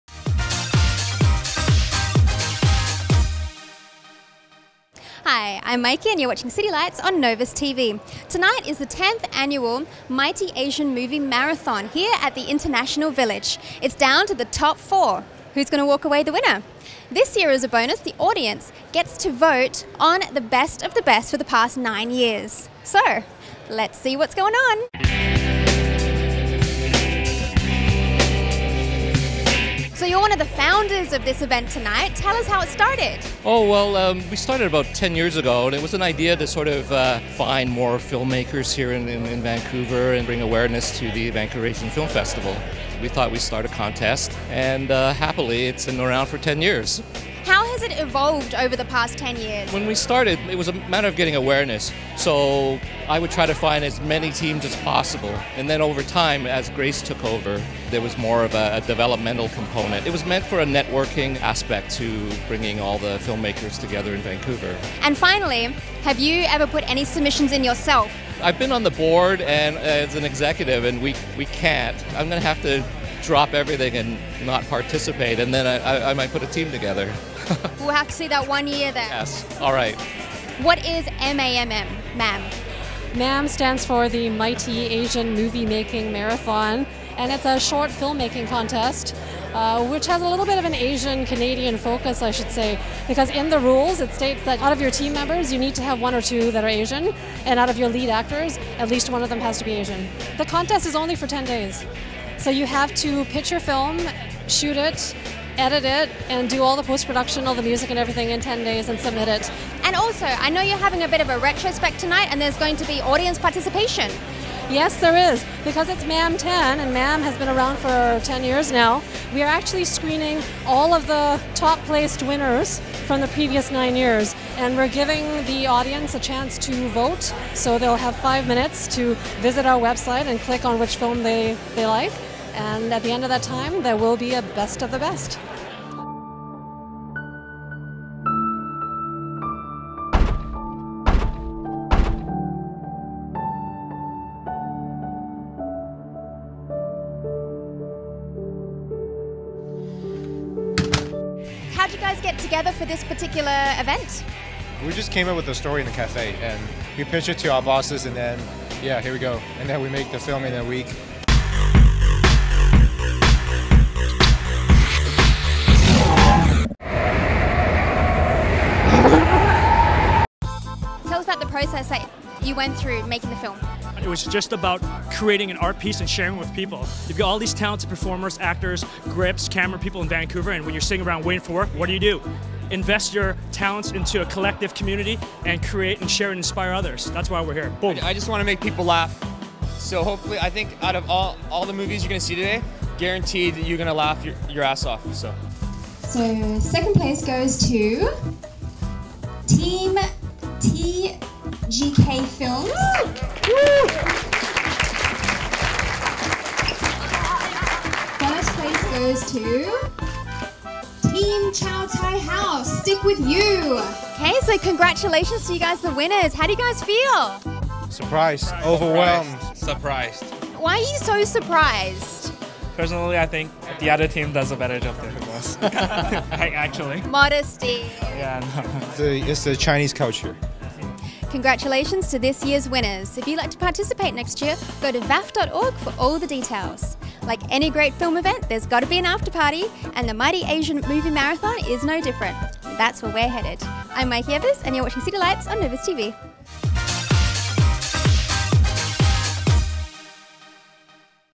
MAMM 10 on Novus TV
Novus TV host Maike Evers visits the tenth annual Mighty Asian Moviemaking Marathon  event. This event brings together the Asian community for an artistic showcase of film talents and creativity. The at the centre of MAMM is a challenge where teams have a time limit of 10 days to put together a movie. Being the 10th year, the festival is inviting the audience to vote the best video out of the winners from the last 10 years.

Vaff.org